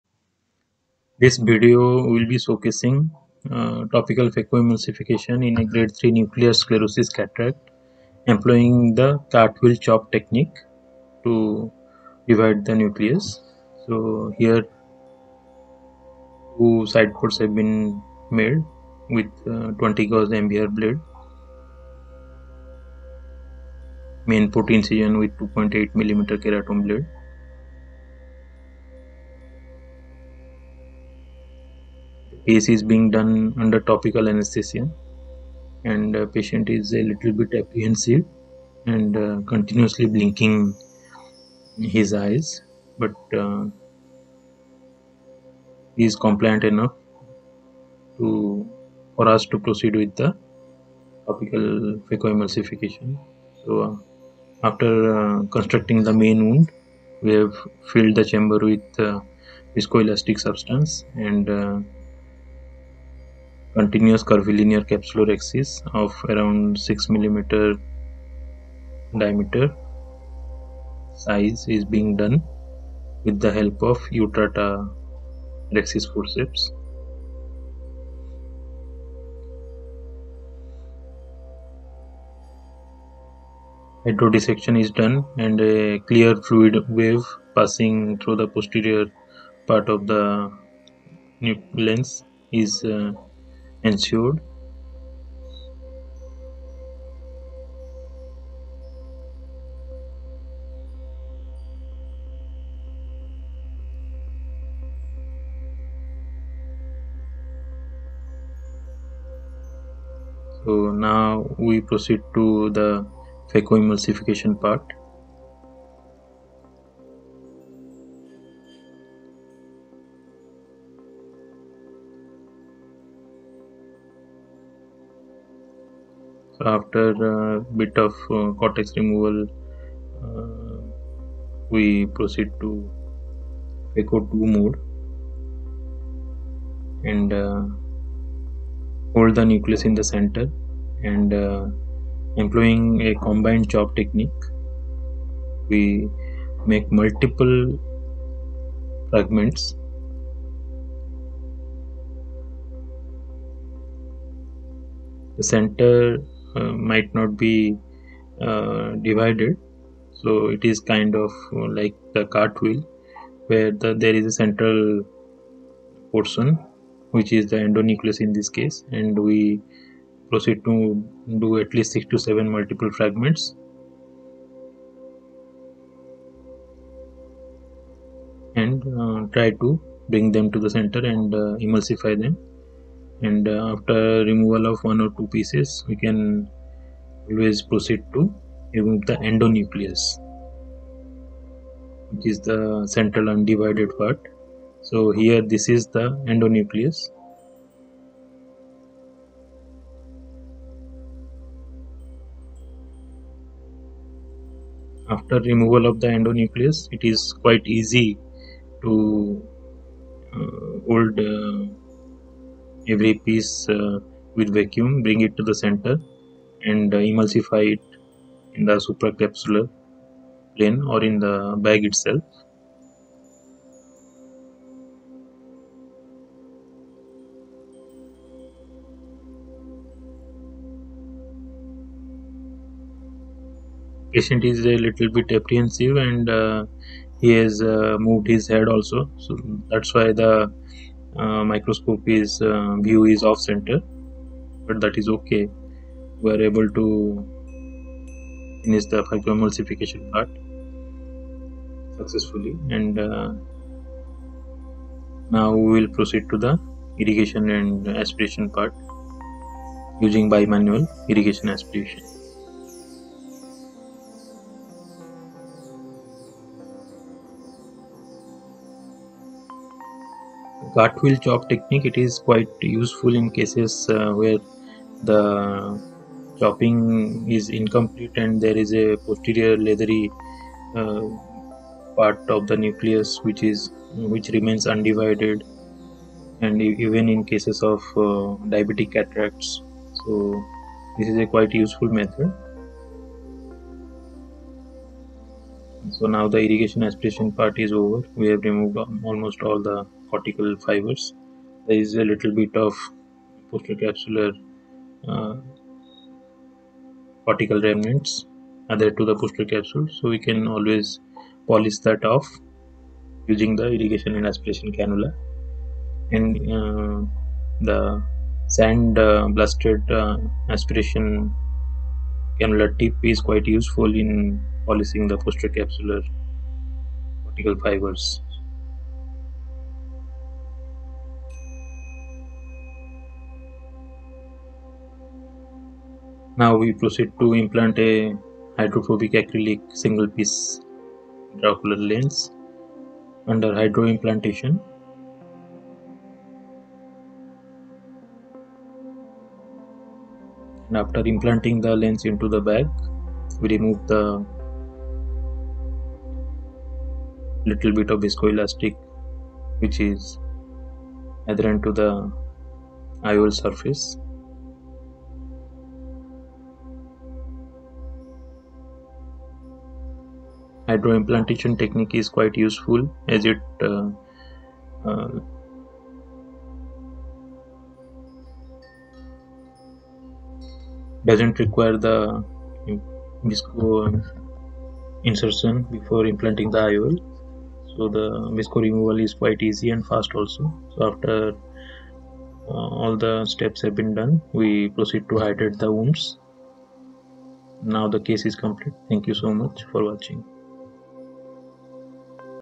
CART-WHEEL CHOP IN TOPICAL PHACOEMULSIFICATION
This video showcases topical phacoemulsification in a grade 3 nuclear sclerosis cataract employing cart-wheel chopping technique for phaco-fragmentation.